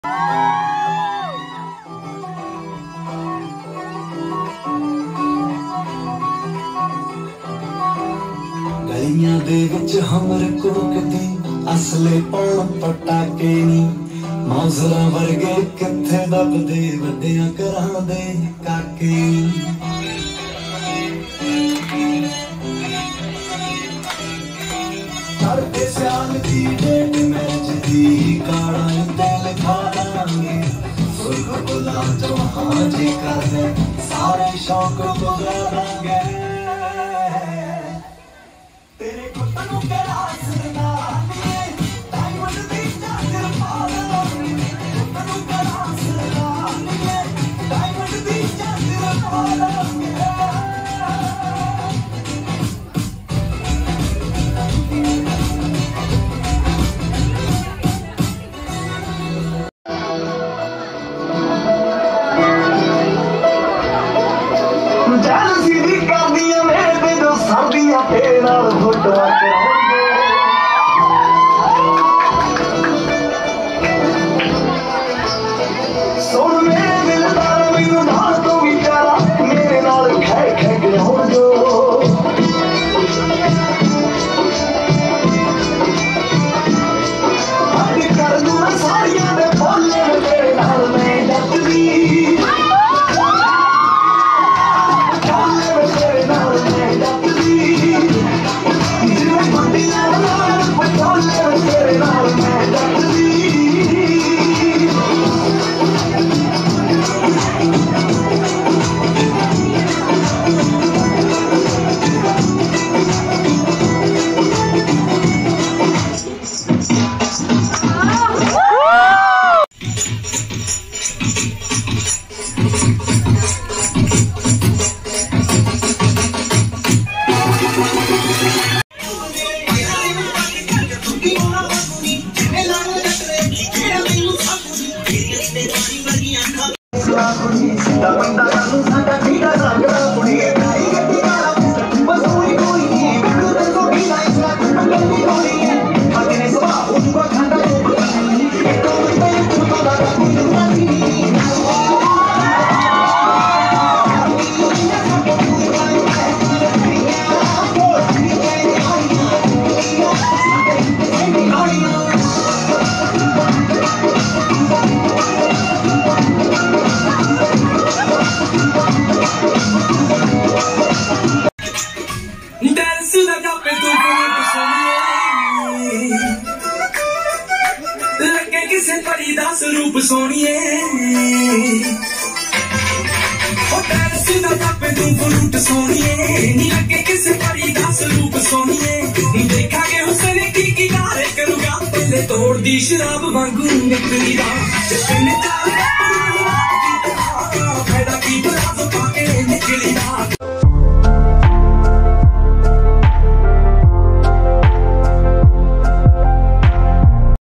Group Dance Performance By MIER College Girls || Mier college jammu||Punjabi Songs Mashup|| Bhangra
Group dance on bhangra
punjabi mashup songs